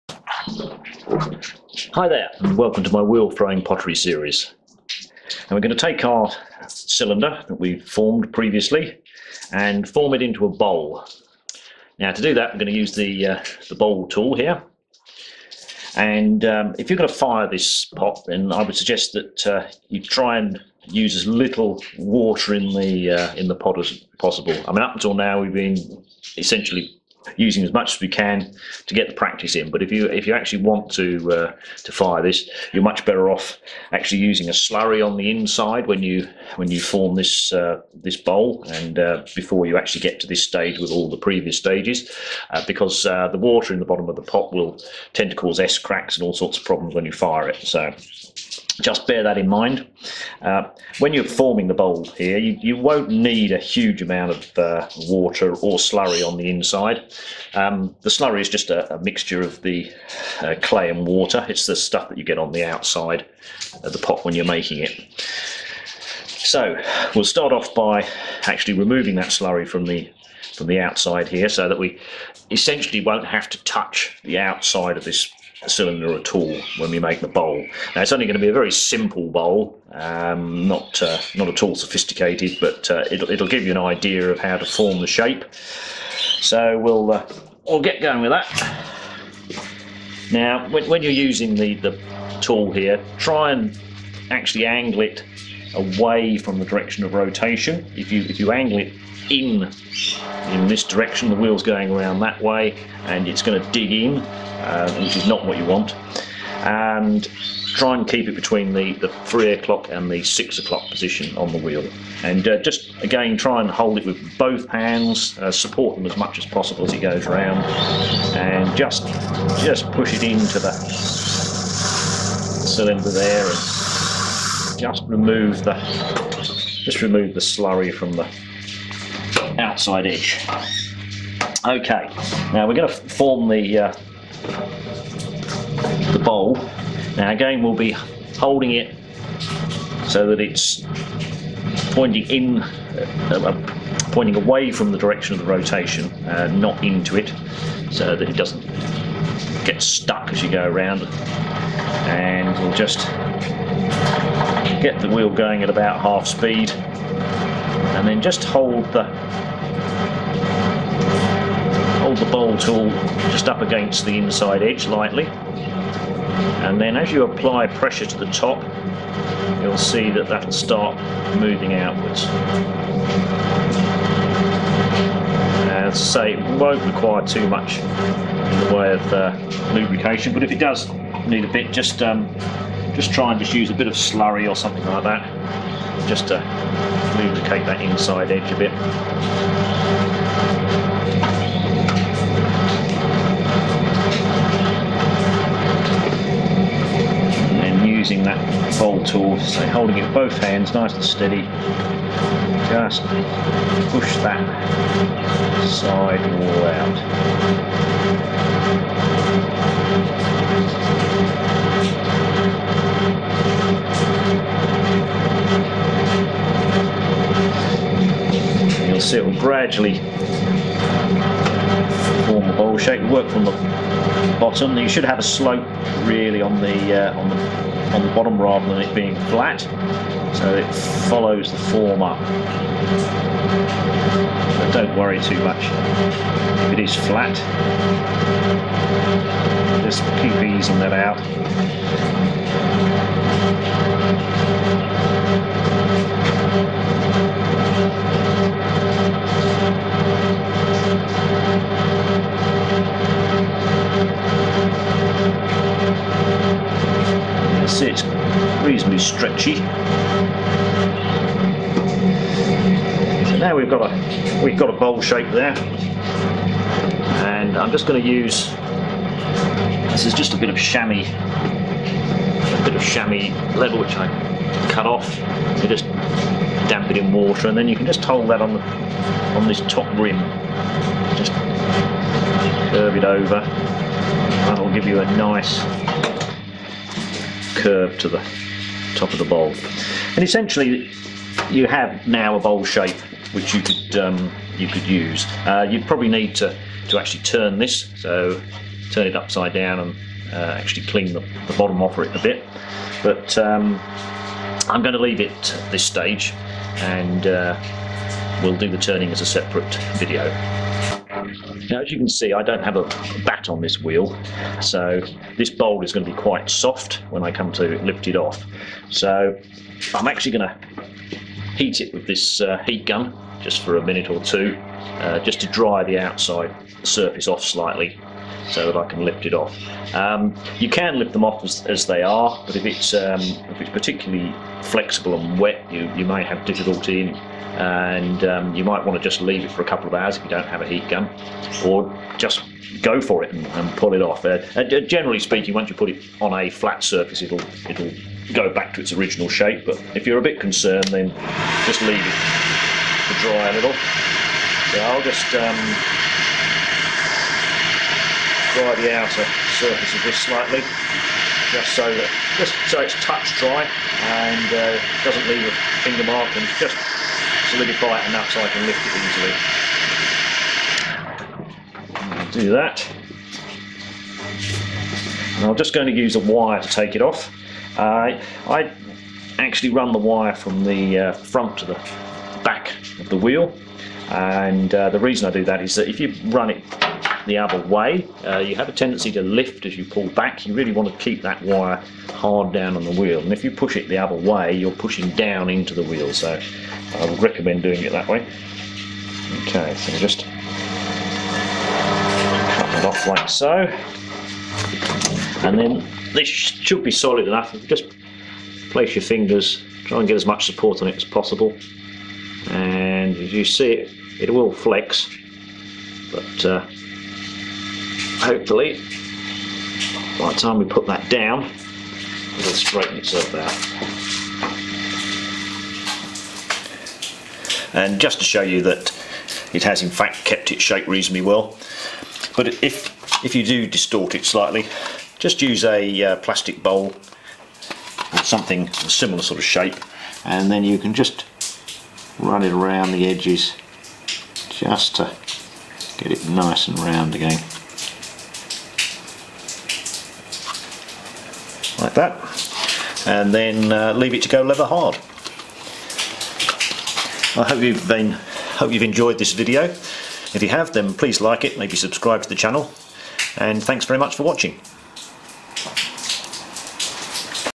Bowl forming for Beginners. Wheel Throwing pottery series.
How to form the bowl shape from a cylinder. Wheel thrown pottery for beginners.